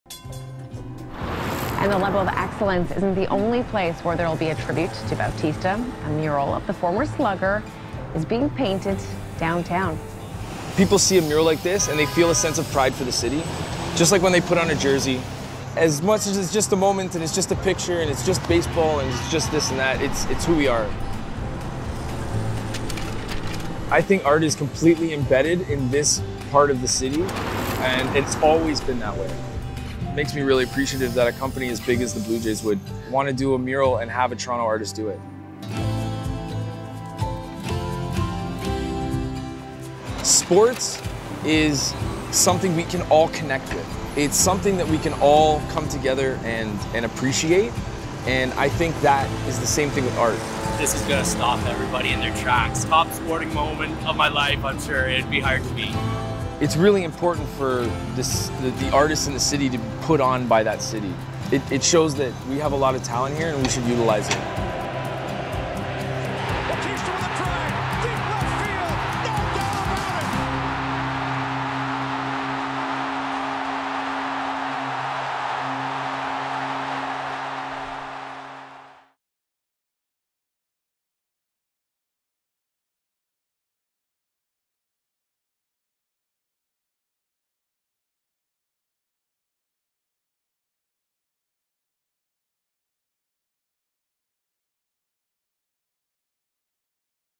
Blue Jays commemorate José Bautista Level of Excellence induction with downtown mural!
To celebrate a special weekend in Toronto, we commissioned a José Bautista mural done by a local artist to celebrate his Level of Excellence induction. This mural, located at Spadina Avenue and Adelaide Street West, will be a fan favourite for years to come!